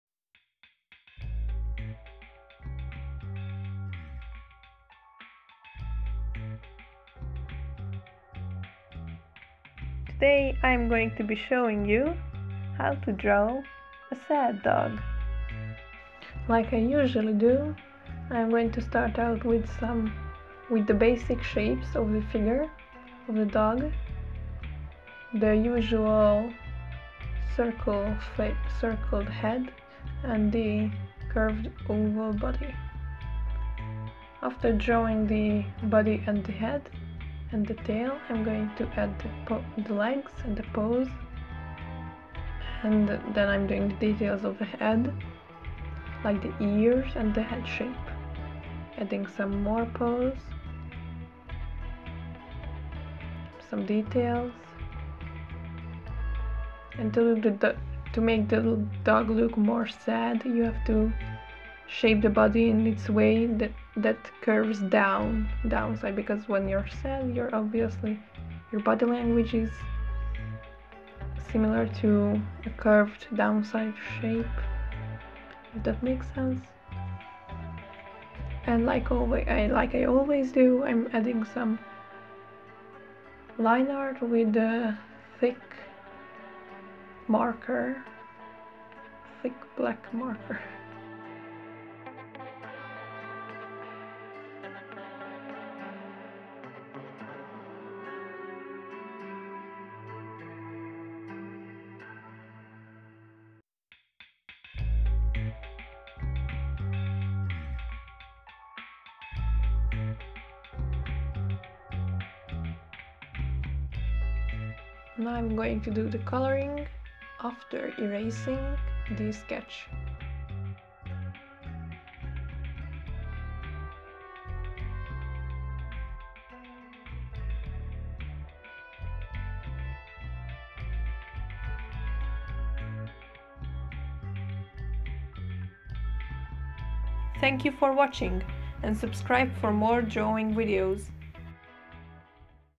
How to draw a sad dog
In this video i'm going to be showing you how to draw a sad dog.